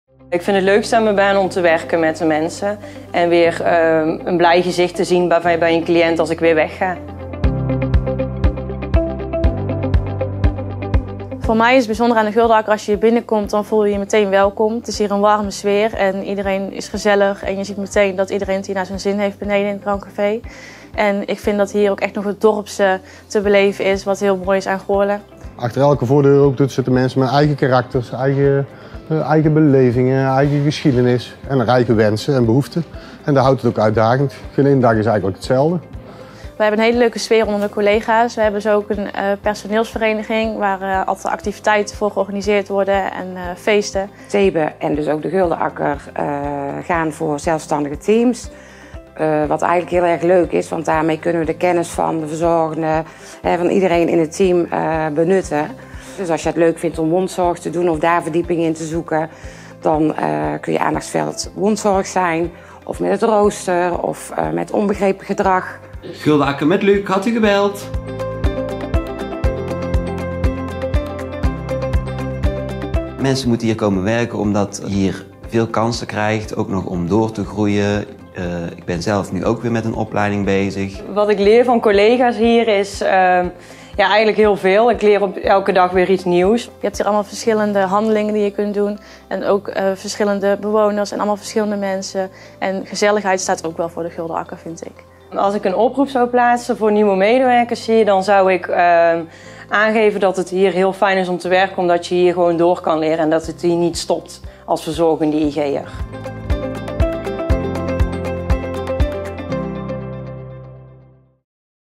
Kom jij werken bij Thebe Guldenakker?
Wil jij komen werken in de zorg? Onze collega's van Thebe Guldenakker vertellen jou graag waarom werken in de zorg zo betekenisvol en leuk is!